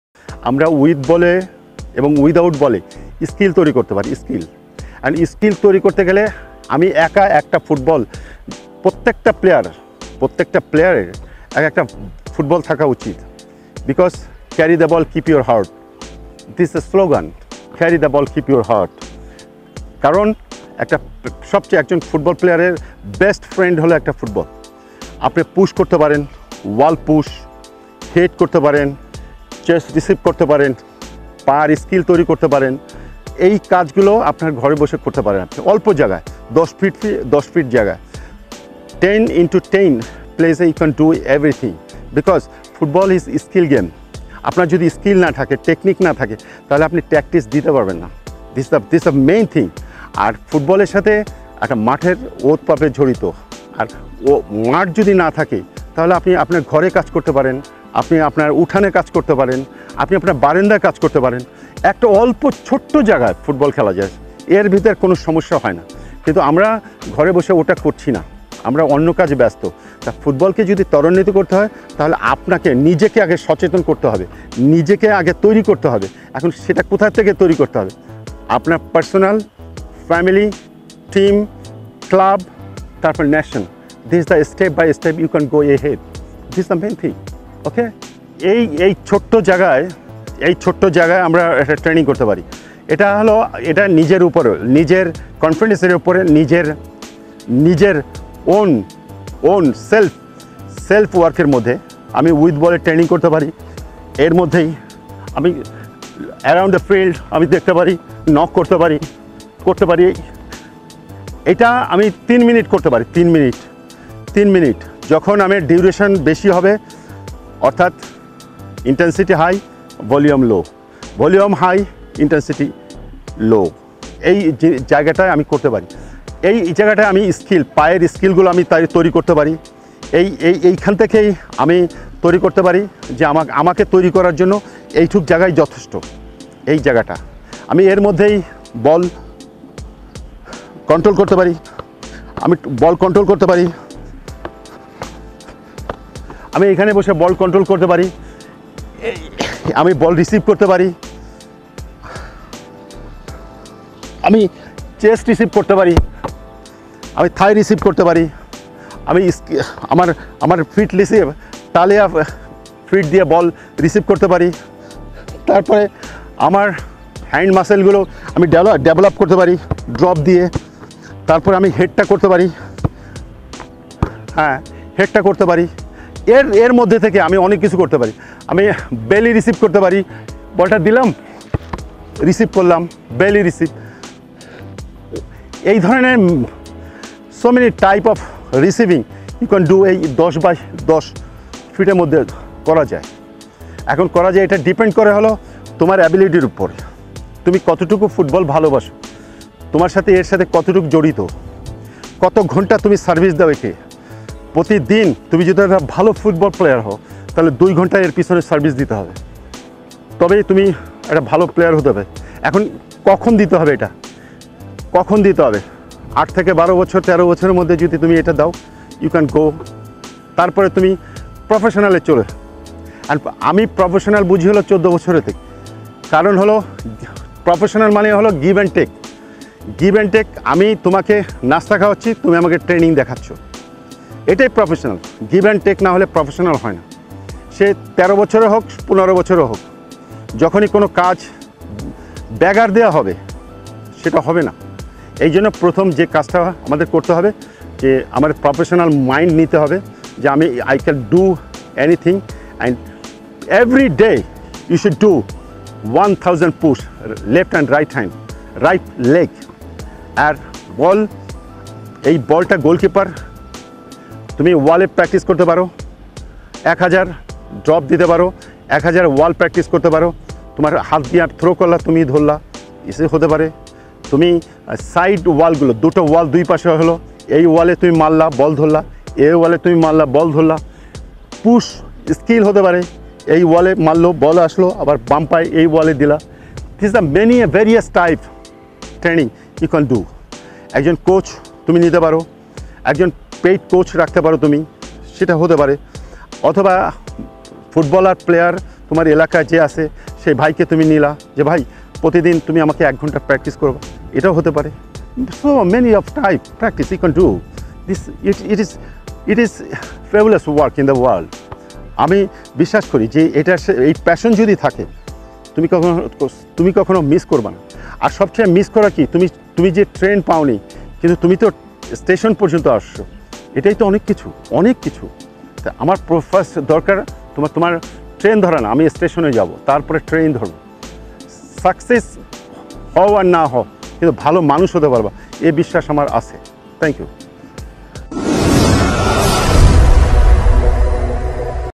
ফুটবল টিপস । Football Tips। With & Without Ball-এ অল্প জায়গাতে কিভাবে স্কিল বাড়াবেন?(SPORTS LIFE TV)
Coach : Abdur Razzak (BFF)
With & Without Ball-এ অল্প জায়গাতে কিভাবে স্কিল বাড়াবেন তা জানালেন কোচ আব্দুর রাজ্জাক..
মাঠে আবারও পুরো দমে ফেরার অপেক্ষায় আছেন ফুটবলাররা। এই সময় অল্প জায়গাতে কিভাবে ফুটবলাররা নিজেদের With & Without Ball-এ অল্প জায়গাতে স্কিল বাড়াতে পারেন তার কিছু কৌশল ও ট্রেনিং জানালেন বাংলাদেশ অতি পরিচিত ও স্বনামধন্য কোচ আব্দুর রাজ্জাক।
ভিডিওটি দেখুন আশা করি কিছু উপকারে আসবে.

A Video by : SPORTS LIFE TV
Directed by : Humayon Shamrat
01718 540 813